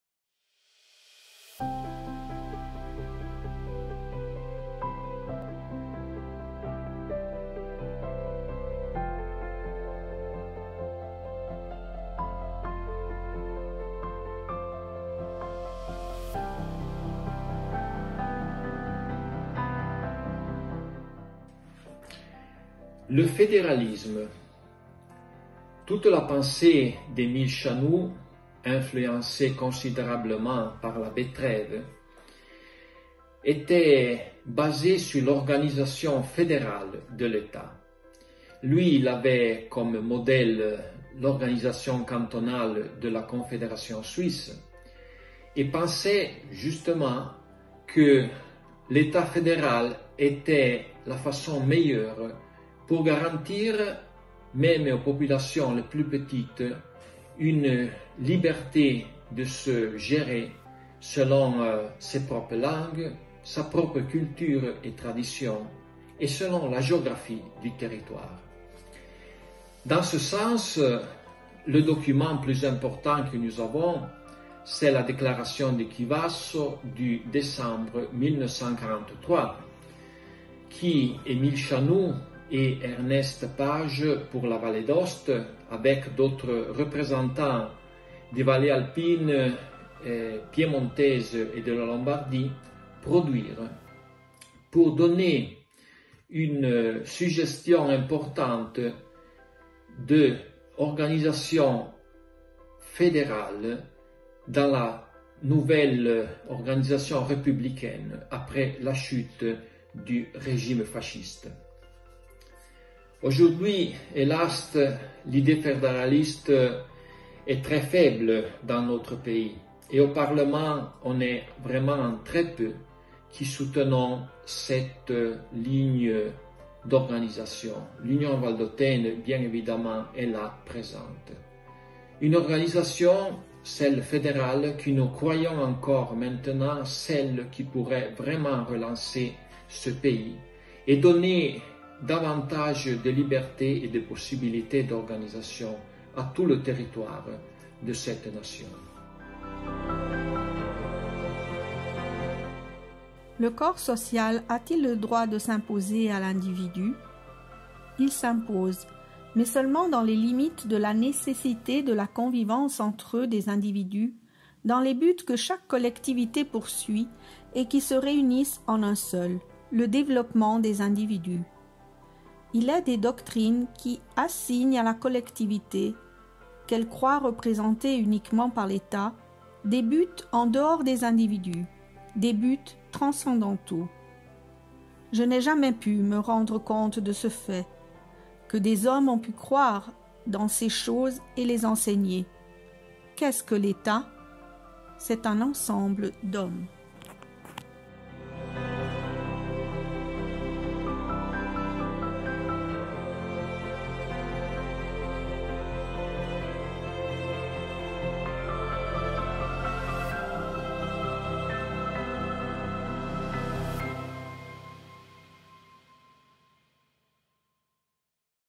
76e anniversaire de la mort d'Émile Chanoux - Le fédéralisme
À l'occasion du 76e anniversaire de la mort d'Émile Chanoux, l'Union Valdôtaine, dans l'impossibilité d'organiser des événements commémoratifs en raison de la situation d'urgence Covid-19, propose des vidéos tout au long de la journée avec des réflexions qui porteront sur l'actualité de sa pensée. Chaque vidéo est accompagnée des lectures tirées de "Les écrits" d'Émile Chanoux.

Dans le cinquième vidéo le Senateur Albert Lanièce aborde le thème du fédéralisme. Lecture de Laurette Proment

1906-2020 N'oublier jamais est notre douvoir.
Ensemble c'est possible